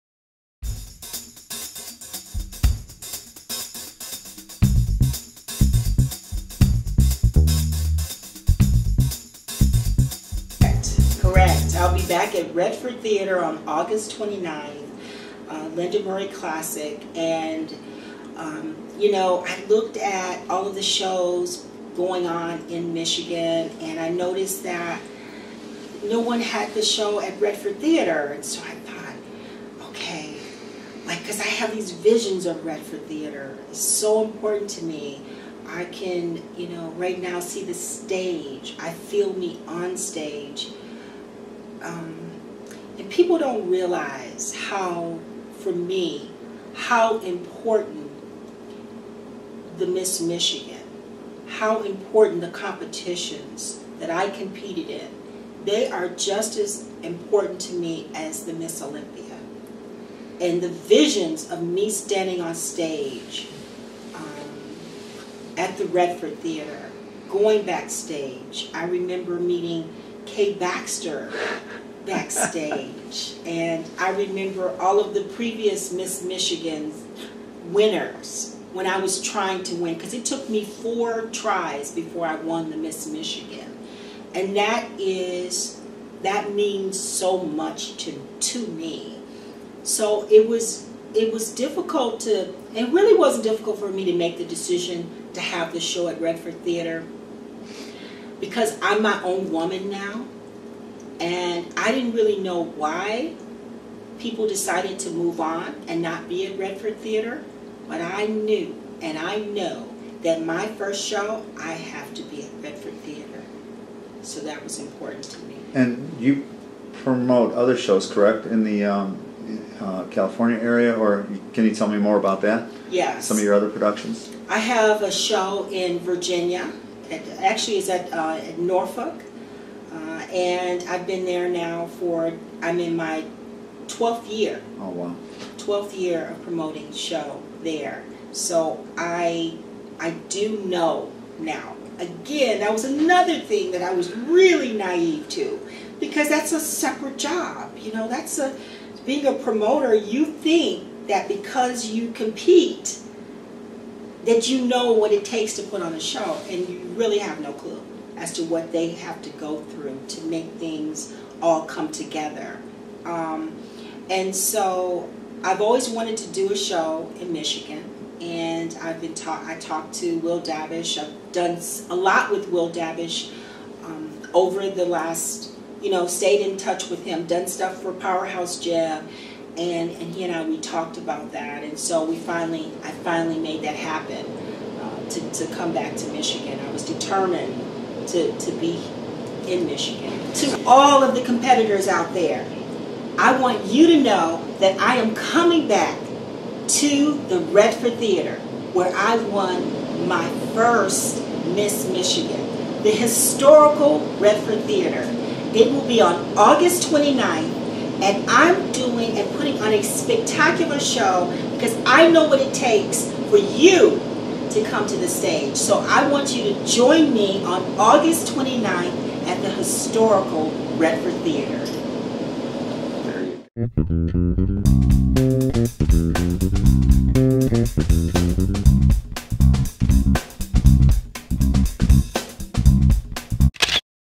2015 Lenda Murray Show Promo Long
The Lenda Murray Classic is coming to the historic Redford Theatre on Saturday August 29, 2015 for a NPC Bikini, Bodybuilding, Figure & Physique Show. Get your tickets now!